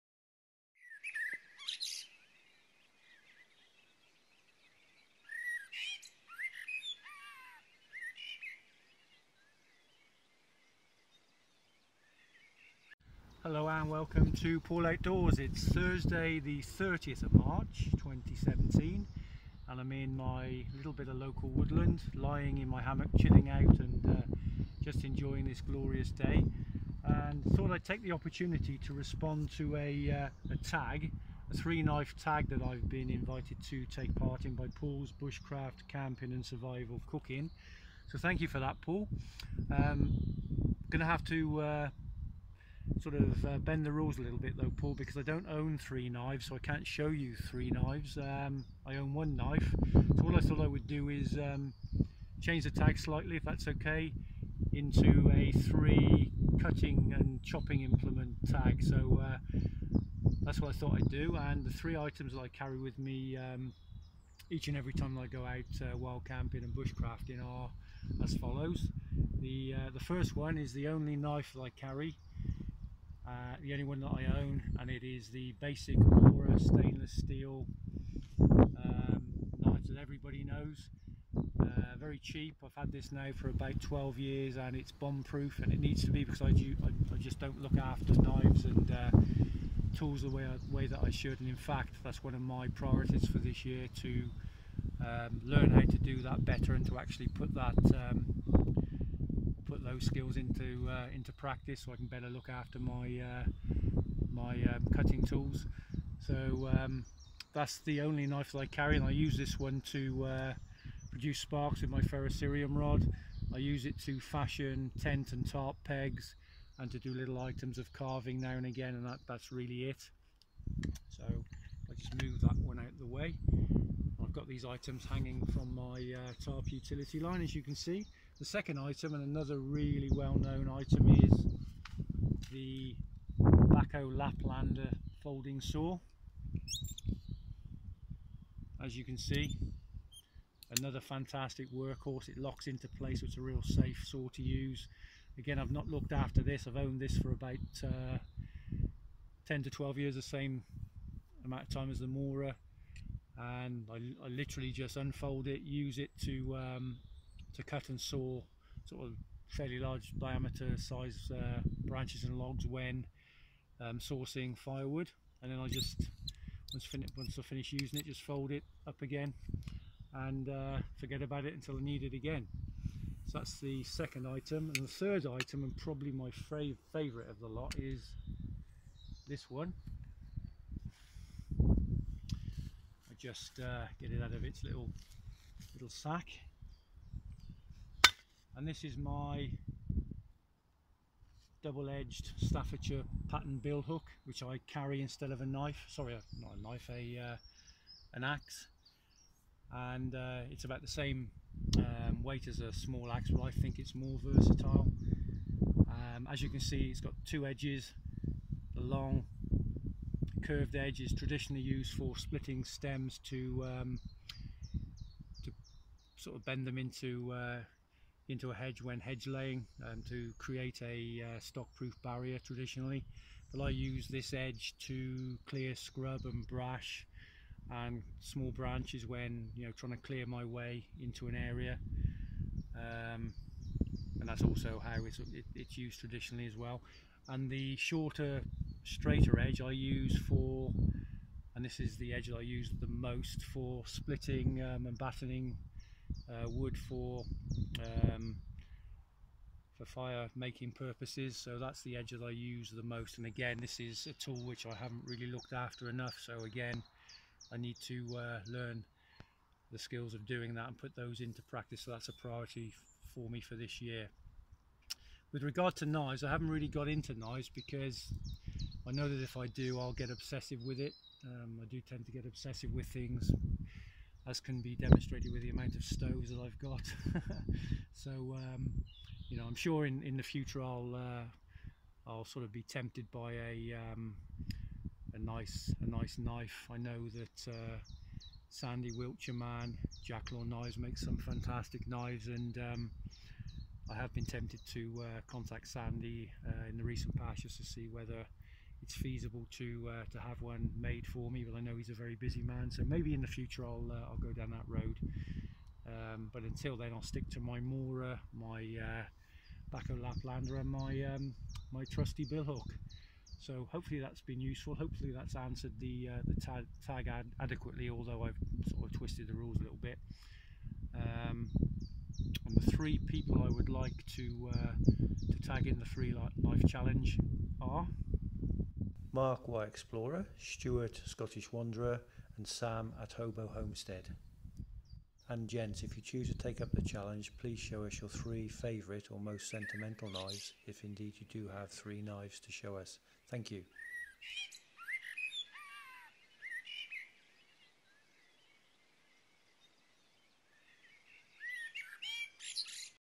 VR Response to Paul's Bushcraft Camping n Survival Cooking 3 Knife Tag
Paul from Paul's Bushcraft Camping n Survival tagged me inviting me to share in a video, my 3 favourite and/or most sentimental knives. Thank you Paul :)

I accepted the invitation but had to bend the rules slightly as I only own and carry one knife, so, I have instead shared my 3 cutting tools that includes my only knife.

To continue the 3 knife tag theme, I would like to tag and invite the following gents to share their favourite and/or sentimental knives:

Mark - YouTube - Wye Explorer
Stuart - YouTube - Scottish Wanderer
Sam - YouTube - Hobo Homestead

Please leave and comments or questions below.

Happy wandering.

Paul :)